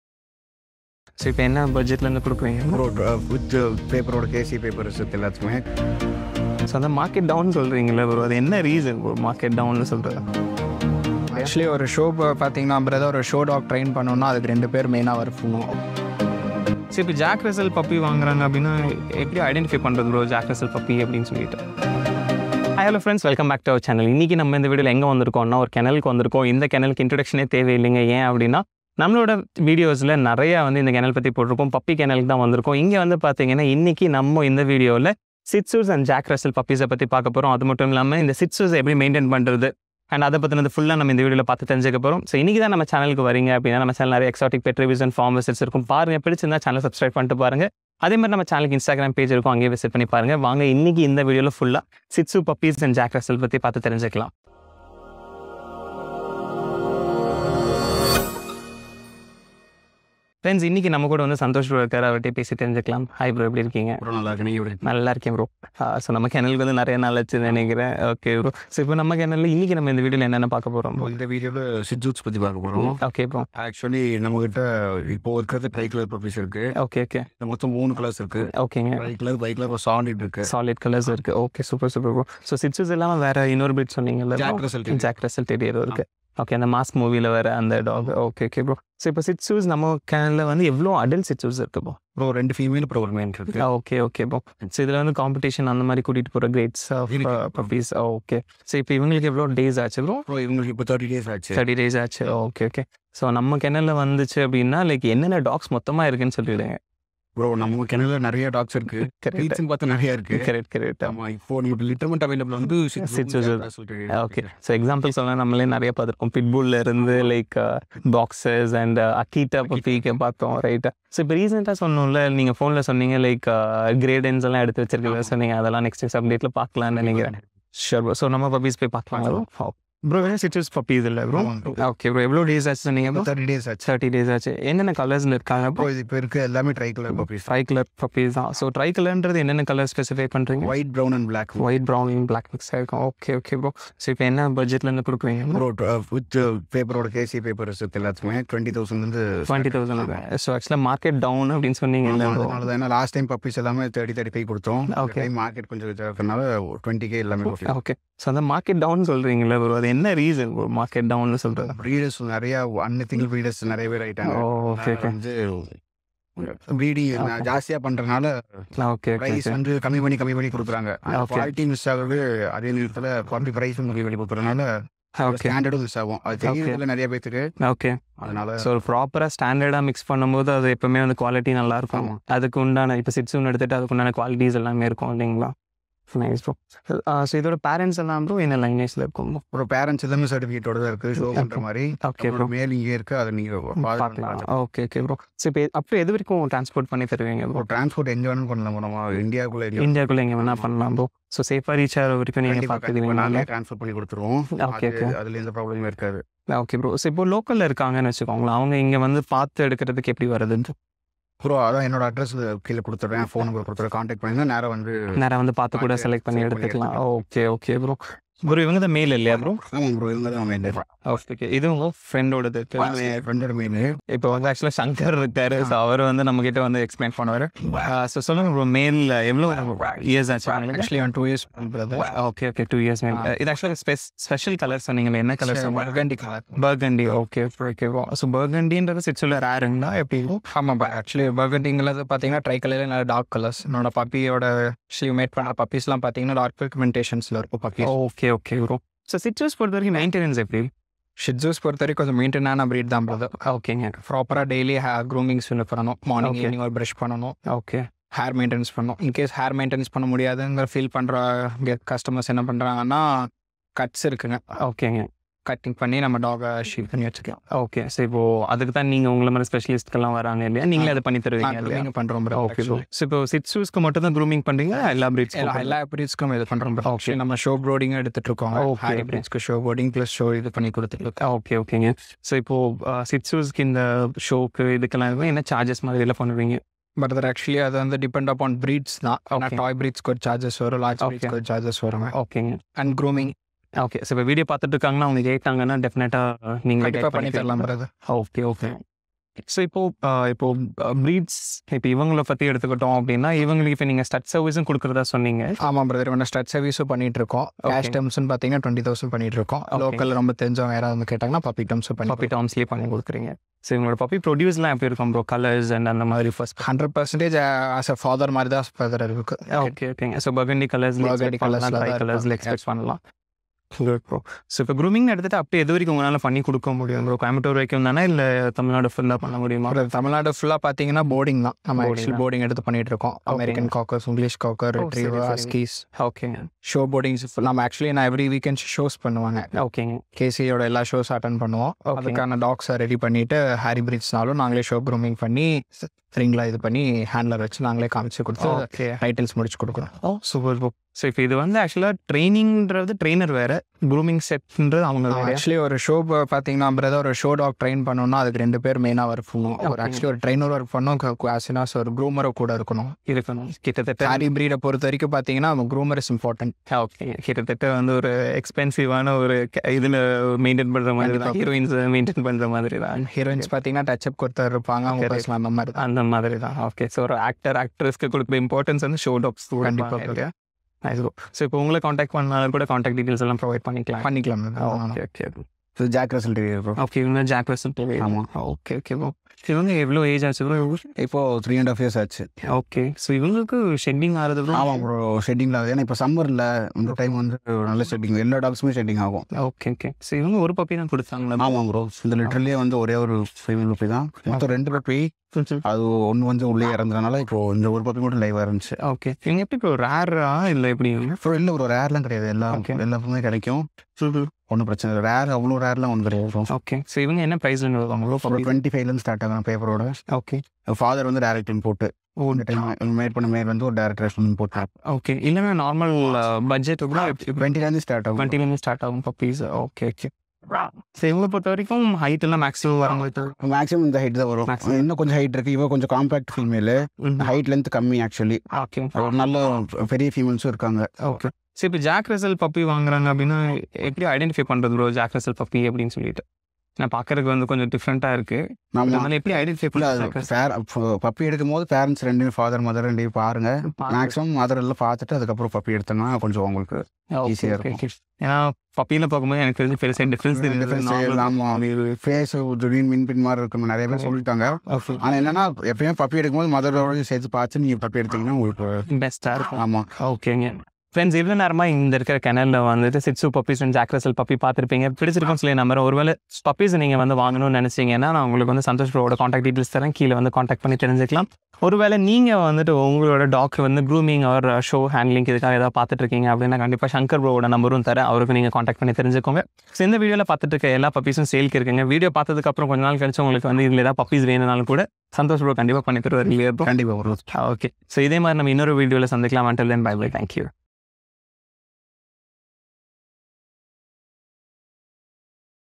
Shihtzu | Jack Russell Terrier | Dogs for sale | Puppy for Sale | Puppy Kennel | Coimbatore Kennel
Hello Friends!

In this video, we visited one of the Coimbatore kennel named "Puppy Kennel"
All Breed Puppies are available for sale
In this video, we cover Shihtzu breed and Jack Russel Terrier breed including puppies sales details

Available Breed Dogs in this kennel
- American Akita
- Pug
- French Bull Dog
- Boxer
- Shih tzu
- Jack Russell Terrier
- Great Dane
Dogs grooming and stud service also available in this place

""Puppy pricing discussed in the video""
Transportation available to all over India

Mr. Santhosh
Ganapathy, Coimbatore

No Animals or Dogs were harmed during making of this video
This video is only for sharing information and knowledge
This video is non-promotional and buying pets on your own risk